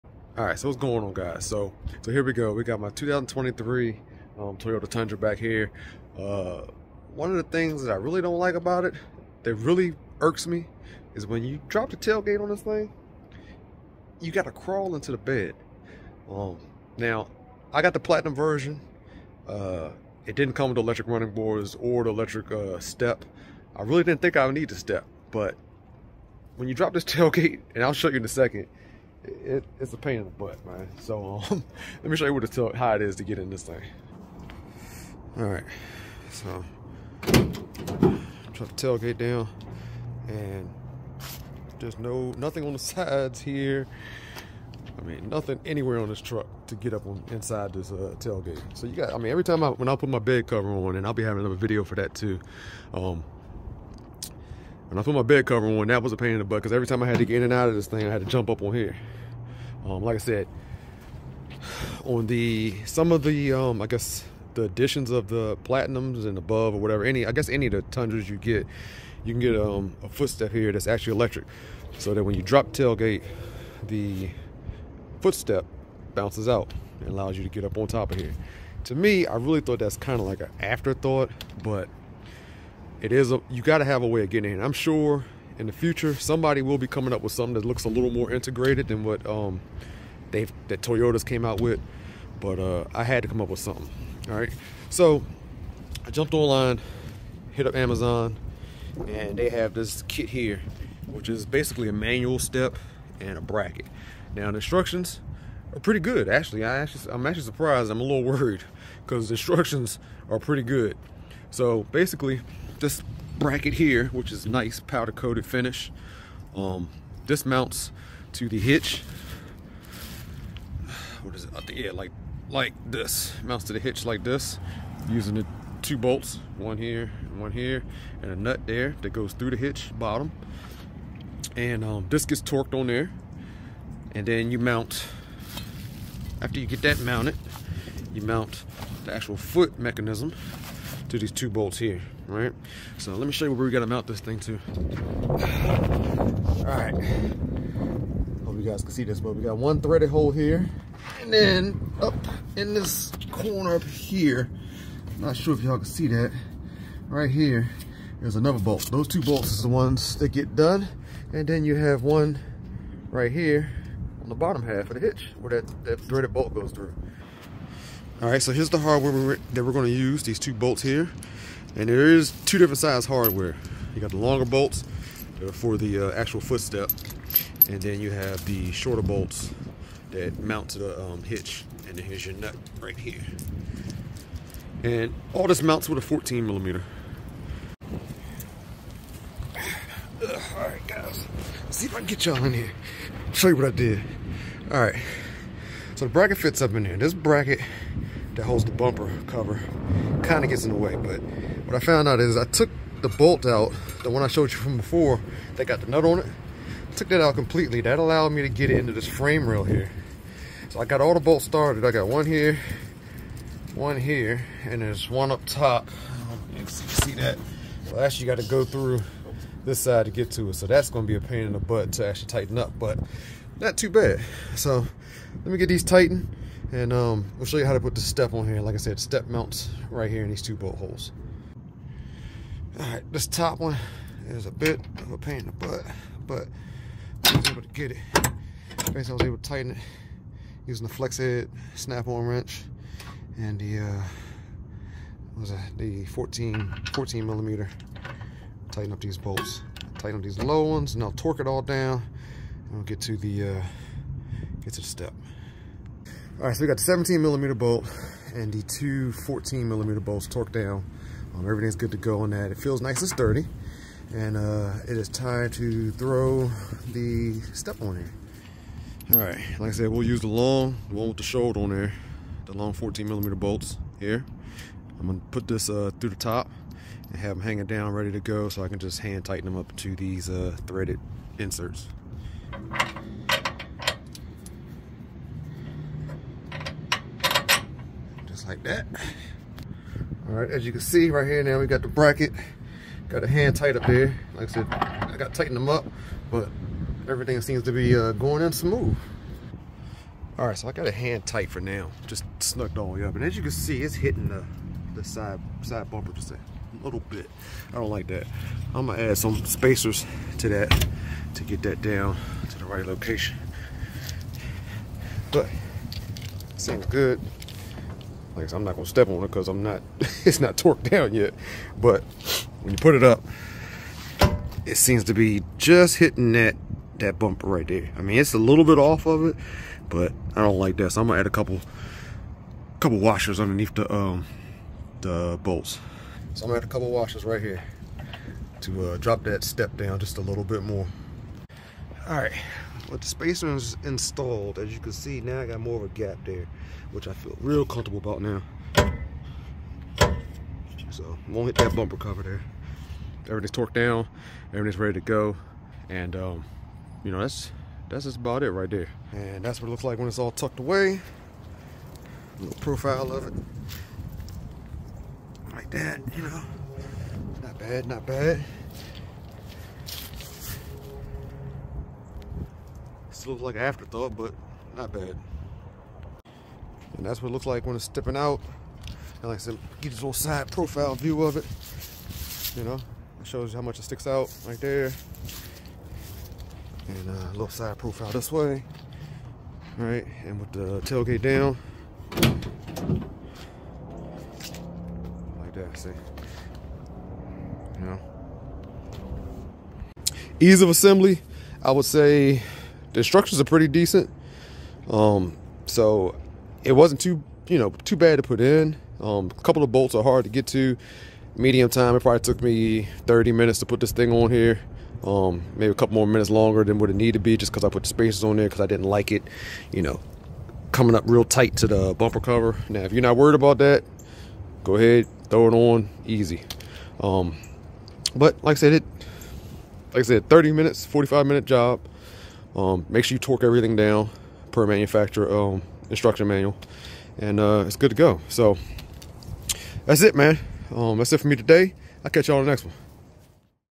2023 Toyota Tundra Bed Tailgate Step Install
Installing the manual step to get in and out of the bed of my 2023 Tundra.